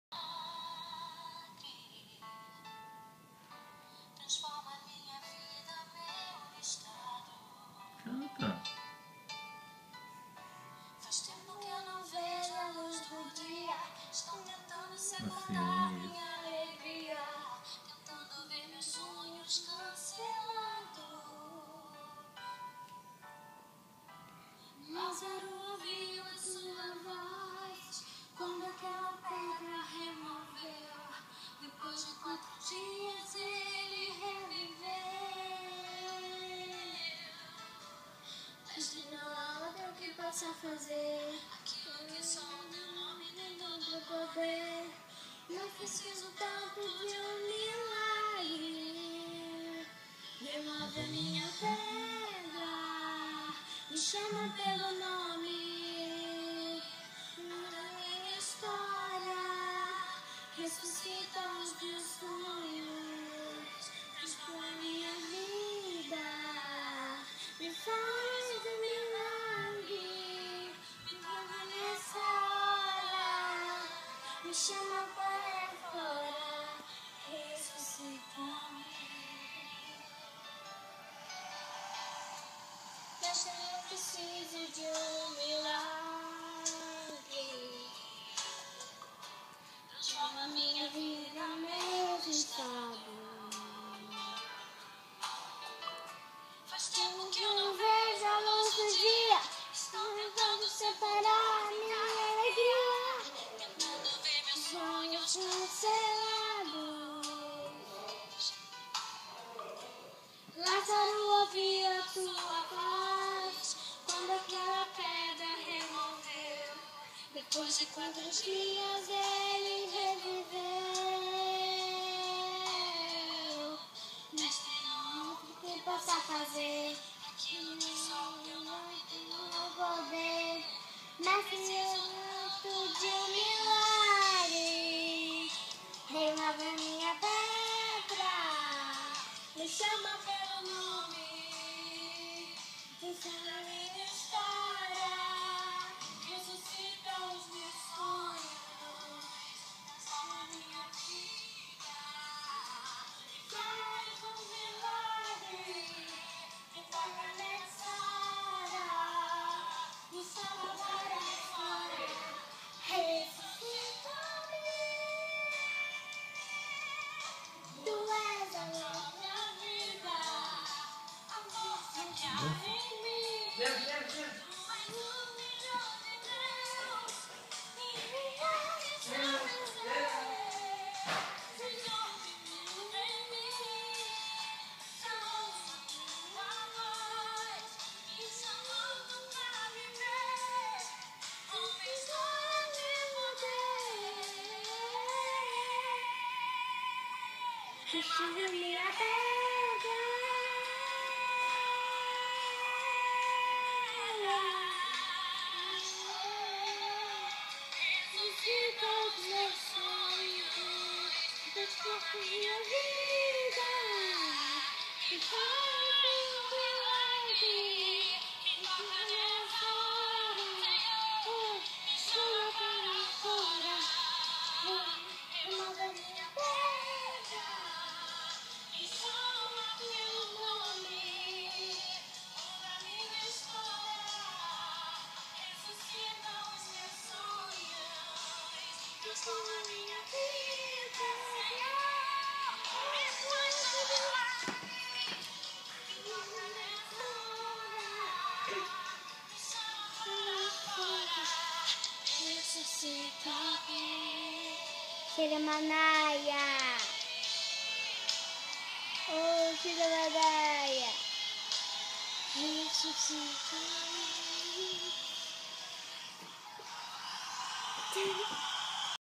Jorge Roque ensina a filha no caminho de Deus!
Catarina Mota Lyrio, 6 anos